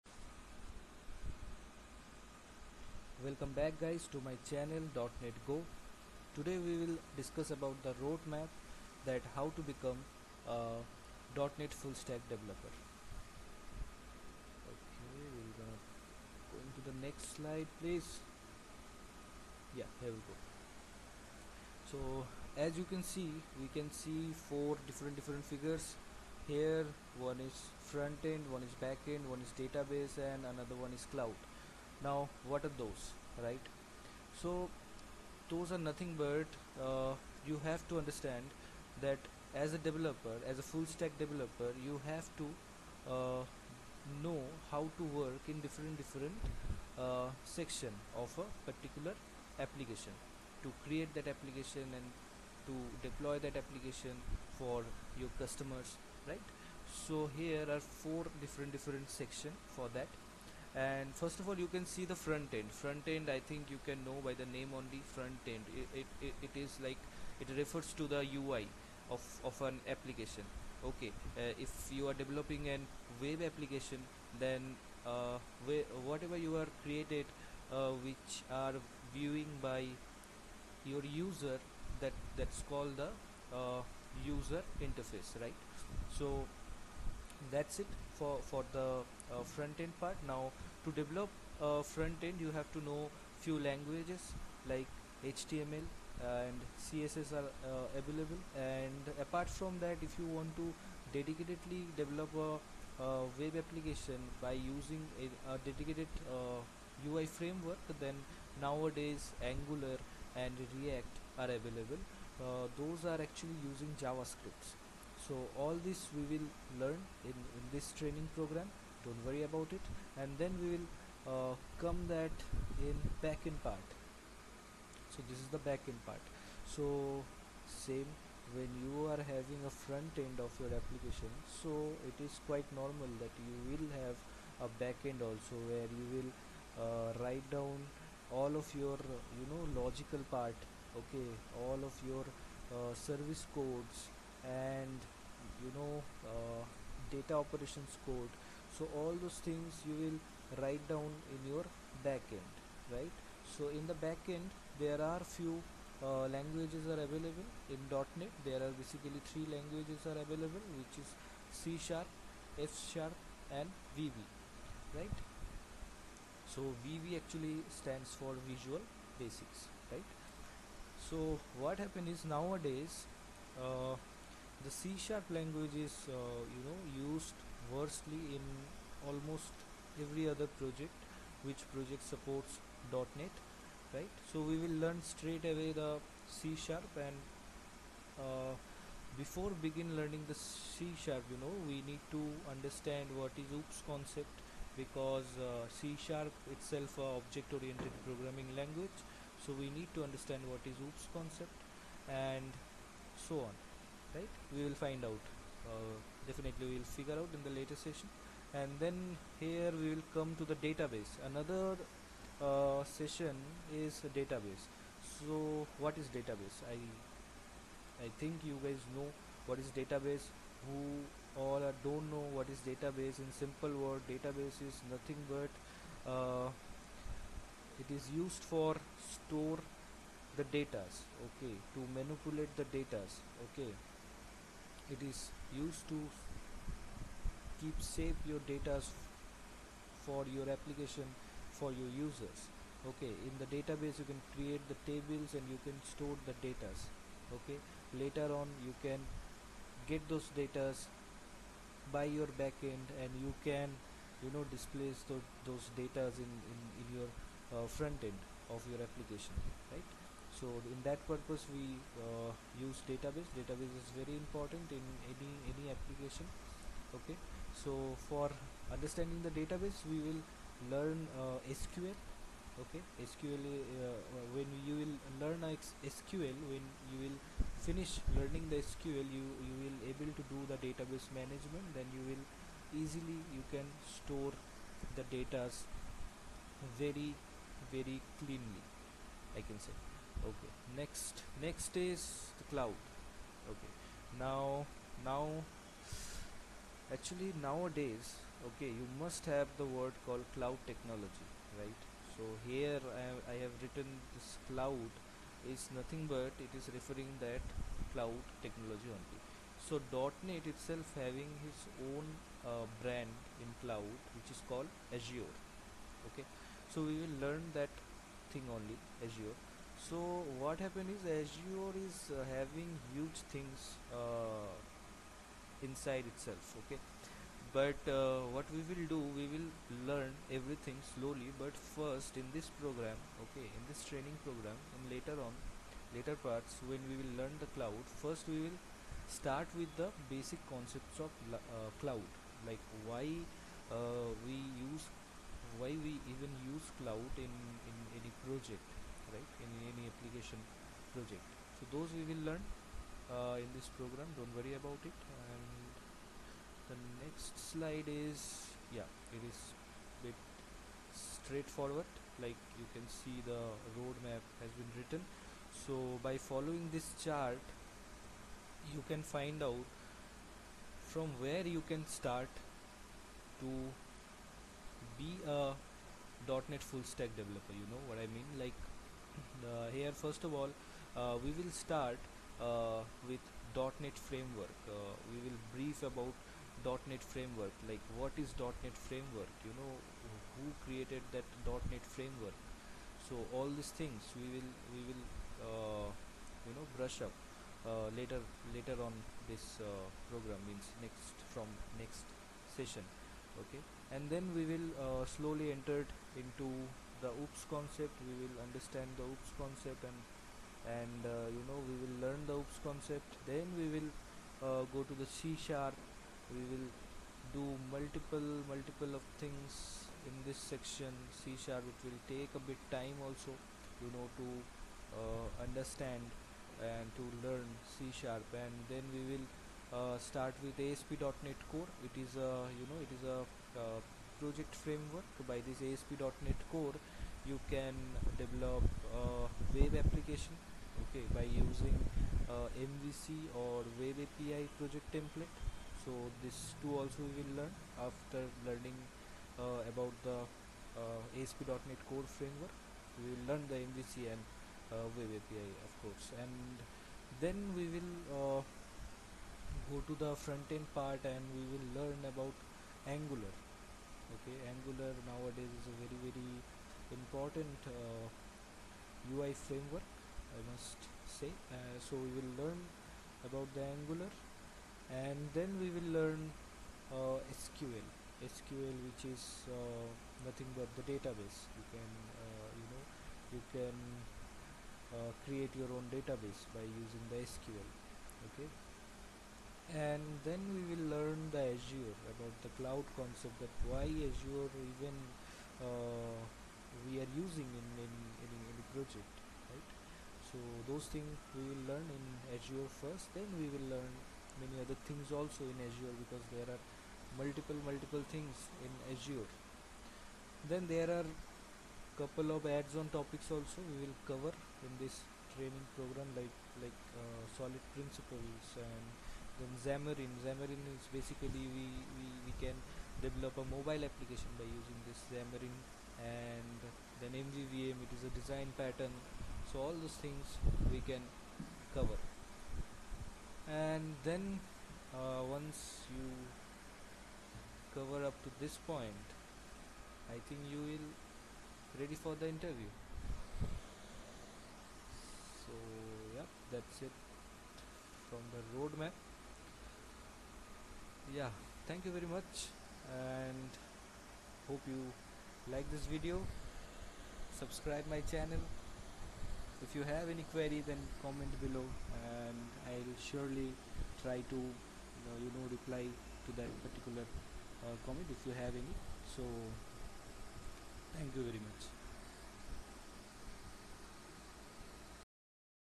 .Net Full Stack Developer Road Map | Induction
This video is containing all the information related to the road map of how to start preparing yourself to be a dotnet full stack developer.